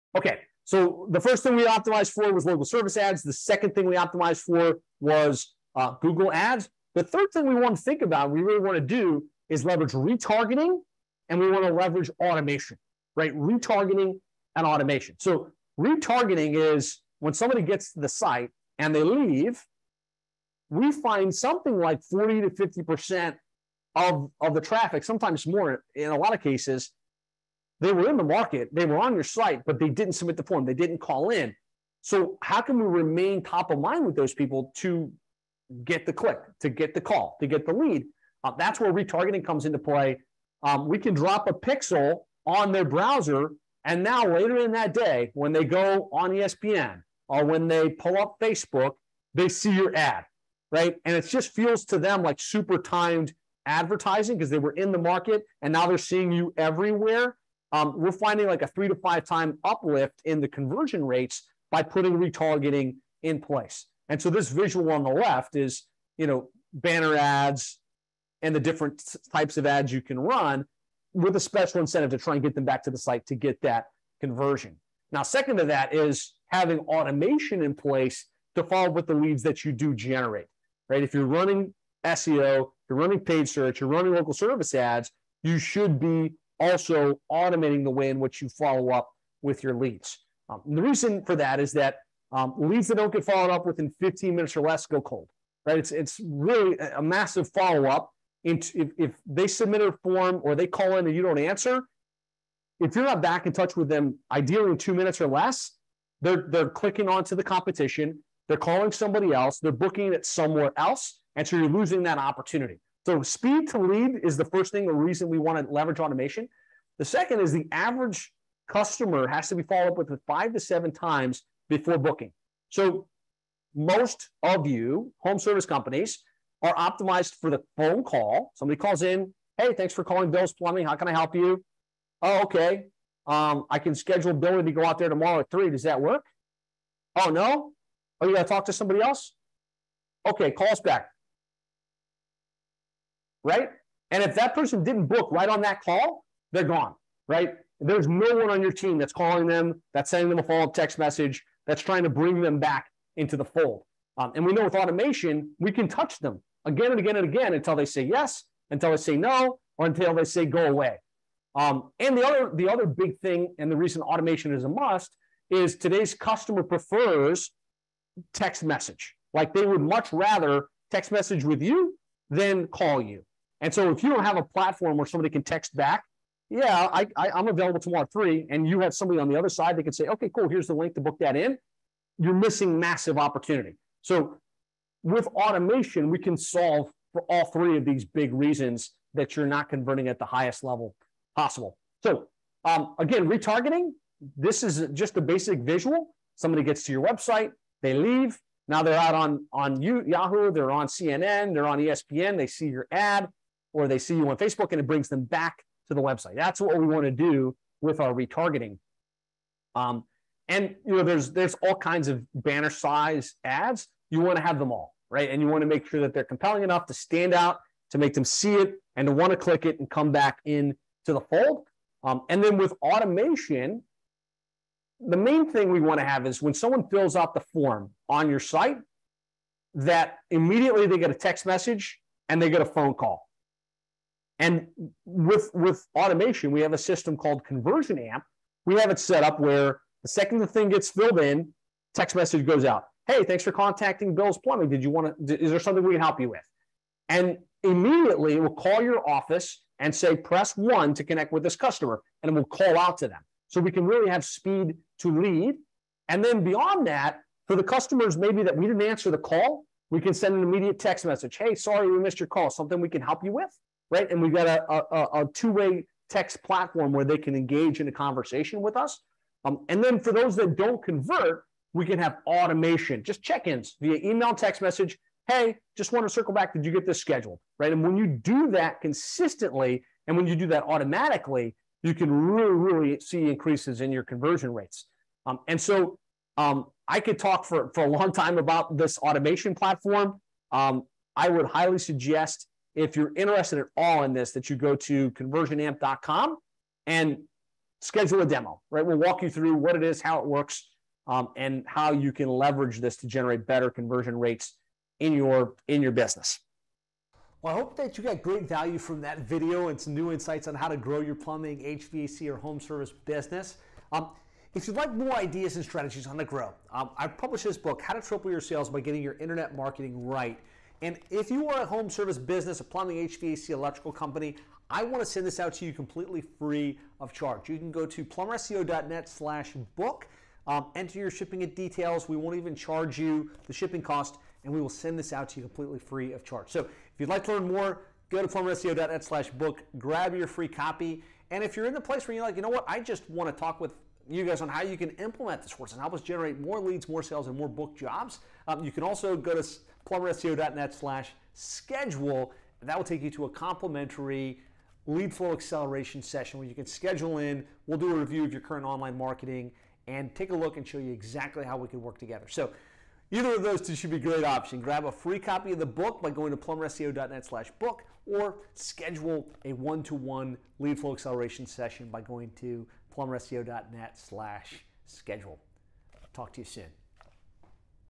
Why Retargeting & Automation for Plumbing, HVAC & Home Service Contractors
As a Plumbing, HVAC or Home Service contractor is ESSENTIAL that you are leveraging Retargeting & Marketing Automation to get the best results from your Online Marketing Investment. In this video we explain why & how.

Grab a free copy of our book How To Triple Your Sales by Getting Your Internet Marketing right

Or